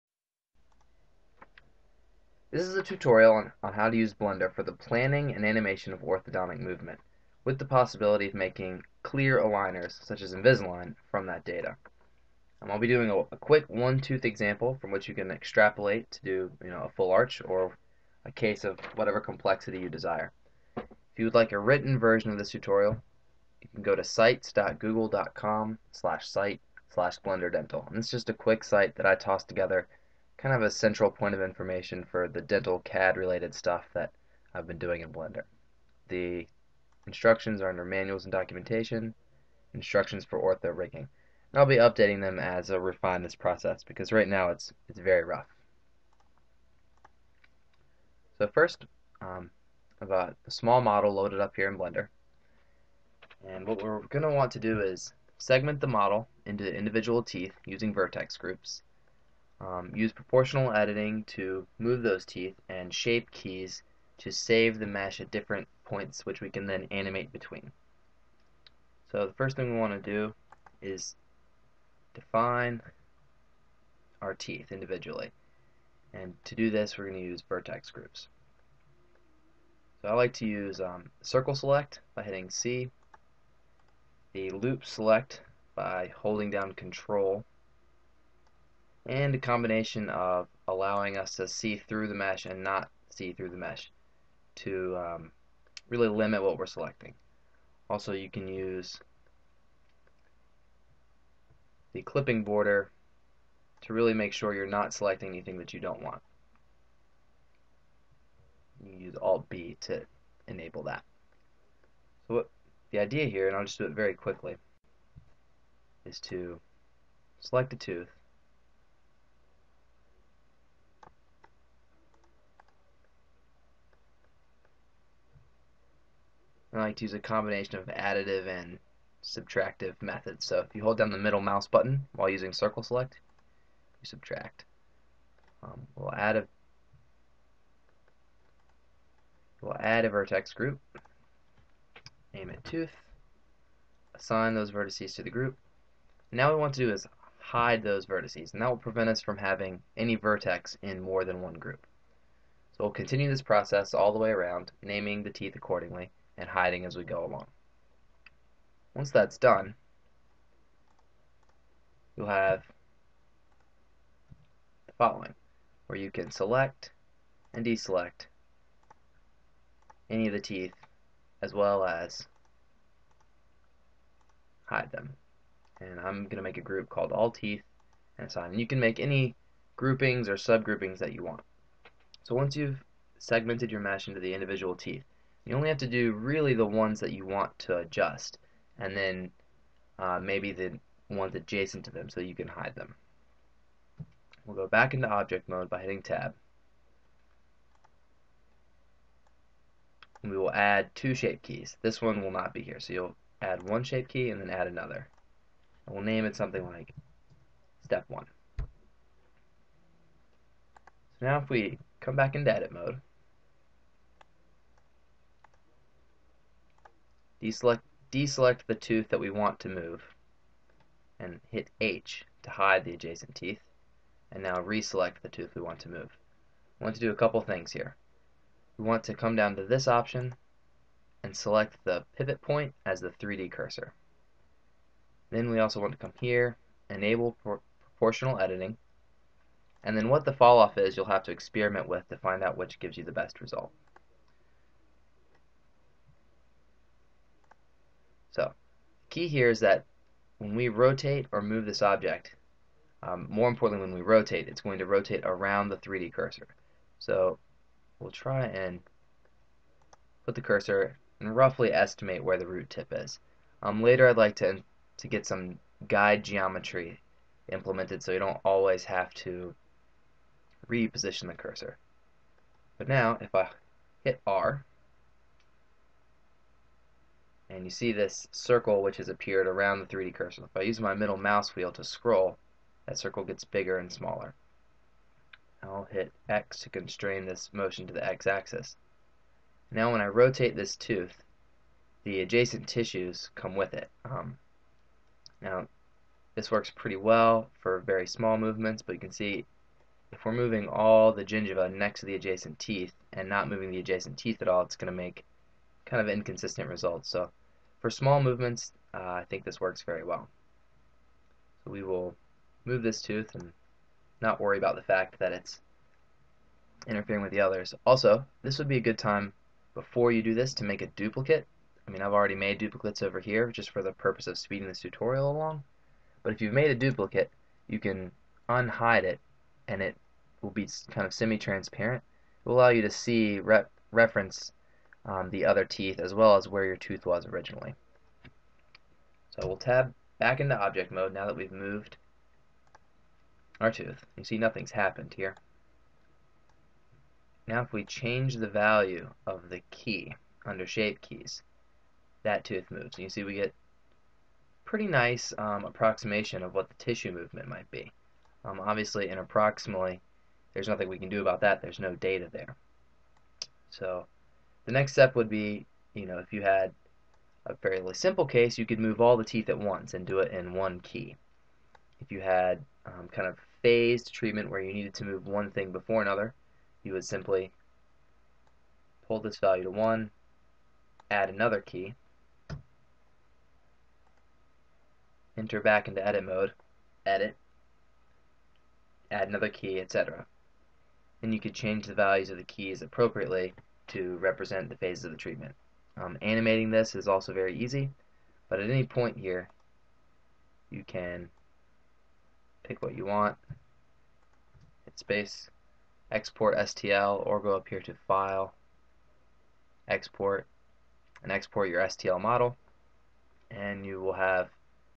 Orthodontic Movement in Blender
How to use blender to plan and visualize orthodontic movement.  A precursor to possibly exporting solid models for the purpose of making a series of clear aligners similar to "invisalign"